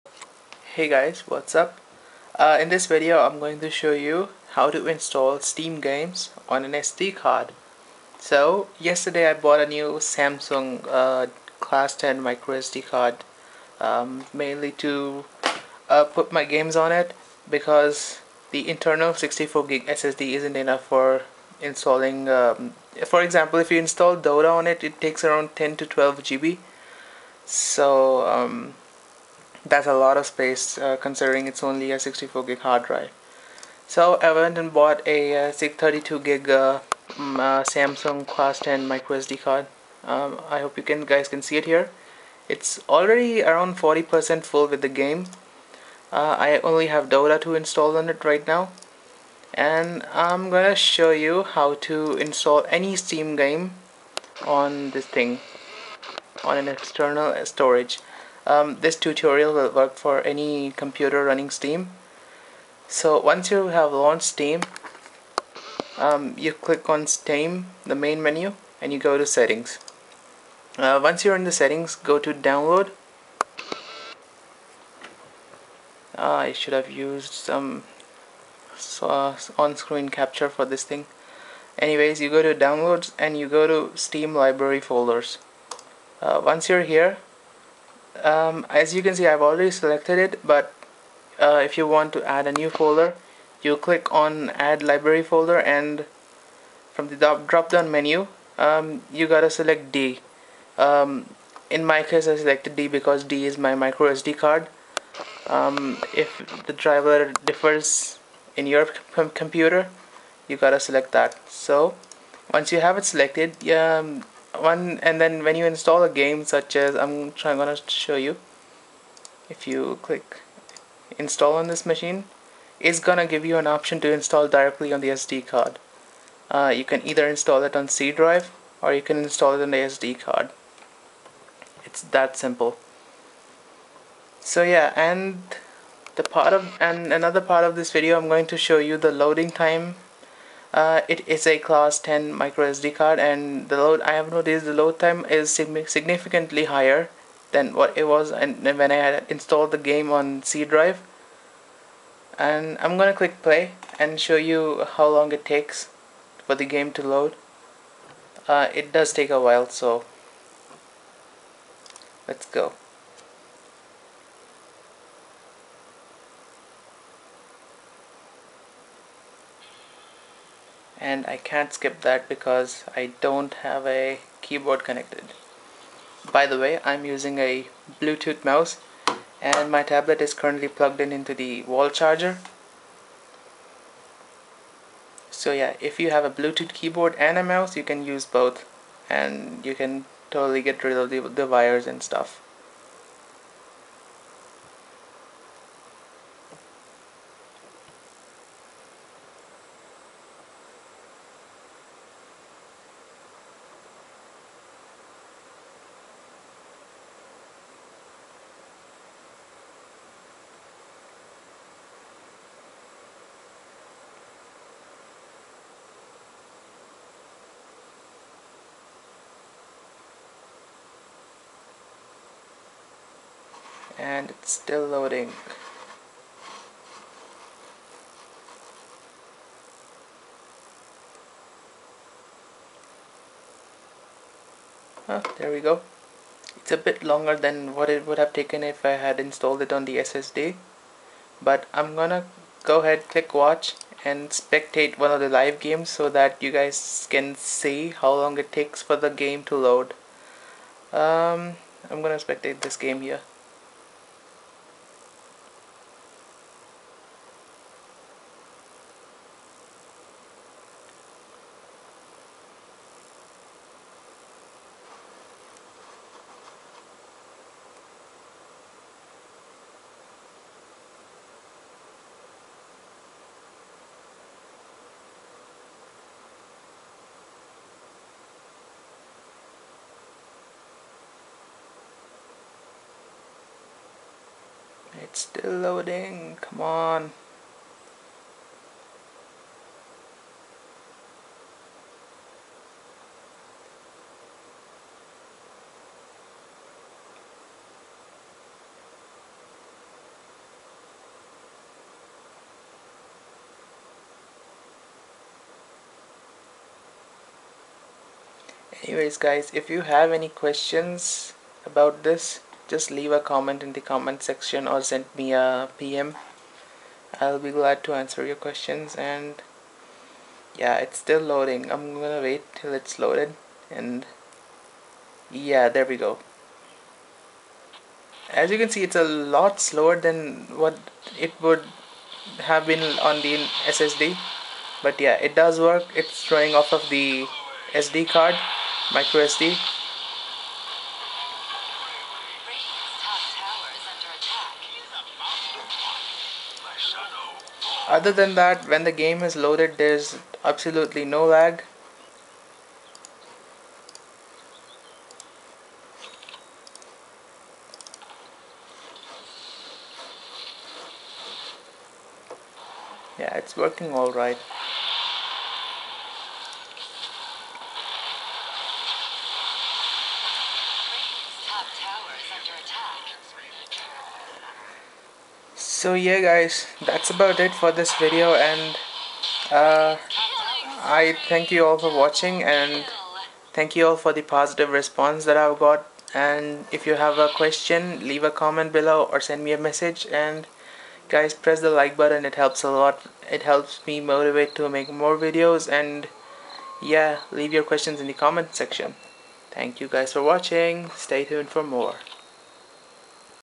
How to install games on micro-sd card (Acer Iconia W4)
Hey guys, here I show you how to configure steam to install games to an Microsd card on my tablet.
You can do the same to ANY program/game while installing just select the install directory to the MicroSDcard.
If you guys have any questions ill be glad to answer them.